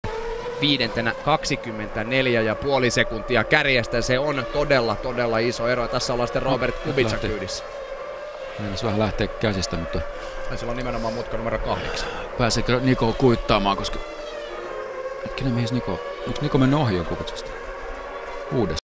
Robert Kubica - Istanbul 2006
Robert Kubica runs wide during the race at Istanbul in 2006
Finnish commentary by Oskari Saari & Erkki Mustakari & Mika Salo